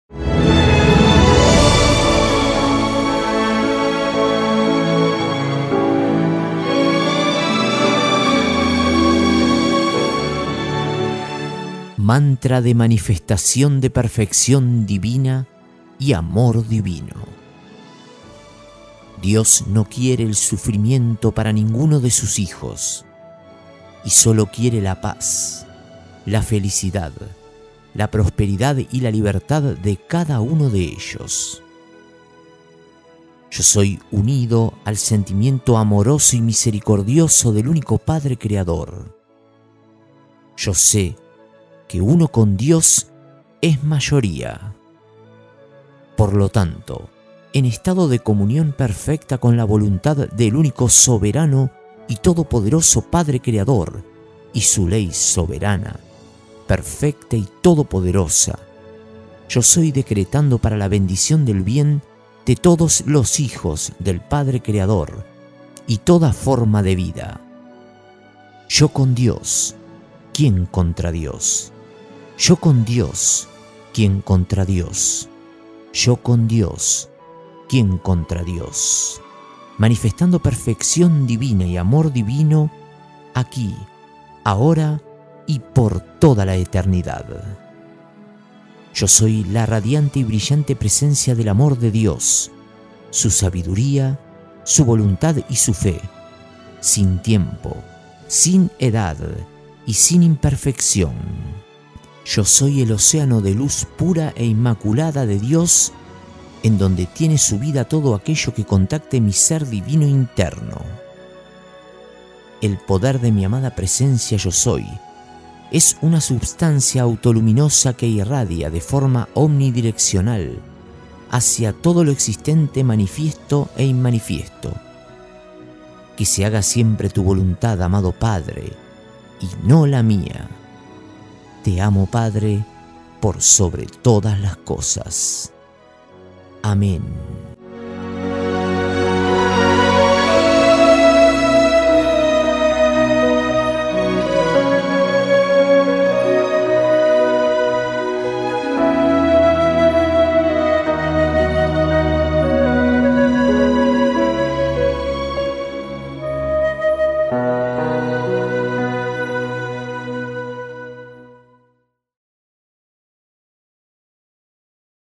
MANTRA DE MANIFESTACION DE PERFECCION DIVINA Y AMOR DIVINO
DIOS NO QUIERE EL SUFRIMIENTO PARA NINGUNO DE SUS HIJOS Y SOLO QUIERE LA PAZ, LA FELICIDAD, LA PROSPERIDAD Y LA LIBERTAD DE CADA UNO DE ELLOS.

YO SOY UNIDO AL SENTIMIENTO AMOROSO Y MISERICORDIOSO DEL UNICO PADRE CREADOR. YO SE QUE UNO CON DIOS ES MAYORIA. POR LO TANTO, EN ESTADO DE COMUNION PERFECTA CON LA VOLUNTAD DEL UNICO SOBERANO Y TODOPODEROSO PADRE CREADOR Y SU LEY SOBERANA, PERFECTA Y TODOPODEROSA, YO SOY DECRETANDO PARA LA BENDICION DEL BIEN DE TODOS LOS HIJOS DEL PADRE CREADOR Y TODA FORMA DE VIDA:

YO CON DIOS QUIEN CONTRA DIOS, YO CON DIOS QUIEN CONTRA DIOS, YO CON DIOS QUIEN CONTRA DIOS, MANIFESTANDO PERFECCION DIVINA Y AMOR DIVINO AQUÍ, AHORA Y POR TODA LA ETERNIDAD.

YO SOY LA RADIANTE Y BRILLANTE PRESENCIA DEL AMOR DE DIOS, SU SABIDURIA, SU VOLUNTAD Y SU FE, SIN TIEMPO, SIN EDAD Y SIN IMPERFECCION.

YO SOY EL OCEANO DE LUZ PURA E INMACULADA DE DIOS, EN DONDE TIENE SU VIDA TODO AQUELLO QUE CONTACTE MI SER DIVINO INTERNO. EL PODER DE MI AMADA PRESENCIA YO SOY ES UNA SUBSTANCIA AUTOLUMINOSA QUE IRRADIA DE FORMA OMNIDIRECCIONAL, HACIA TODO LO EXISTENTE MANIFIESTO E INMANIFIESTO.

QUE SE HAGA SIEMPRE TU VOLUNTAD AMADO PADRE Y NO LA MIA.

TE AMO PADRE POR SOBRE TODAS LAS COSAS.

AMEN.

ERIC